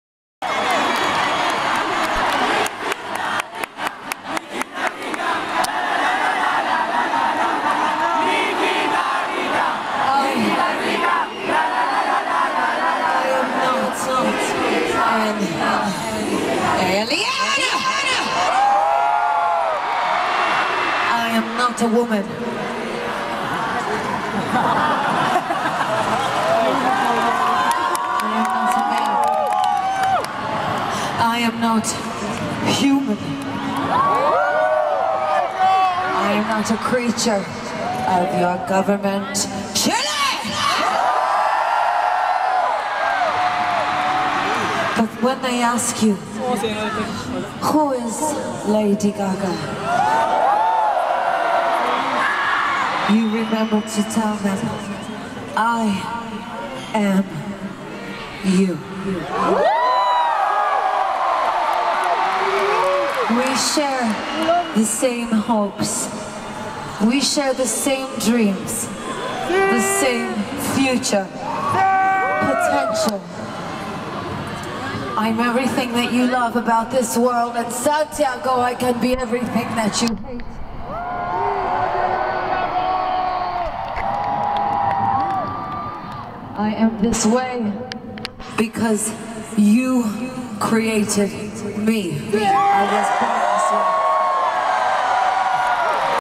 Lady Gaga en Chile, I am You!
I am not an alien,I am not a woman, I am not a man, I am not a human, I am not a creature of your government Chile! , and when they ask you Who is Lady Gaga, you remember to tell them , I am you.